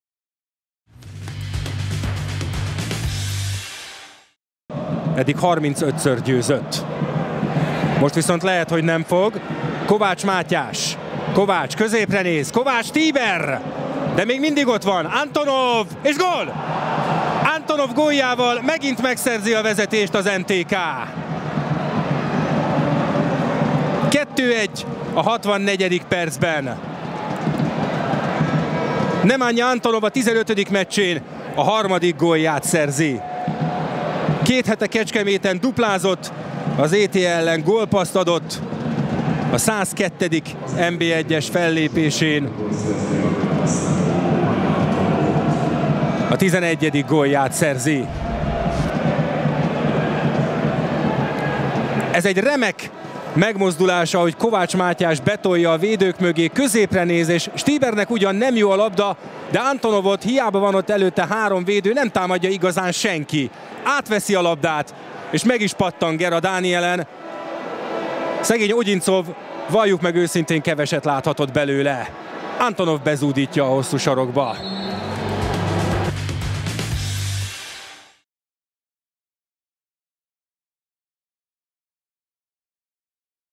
Nemanja Antonov gólja a DVTK - MTK Budapest mérkőzésen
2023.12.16. OTP Bank Liga, 17. forduló
DVTK - MTK Budapest | 3-3 | (1-1) |
Nemanja Antonov gólja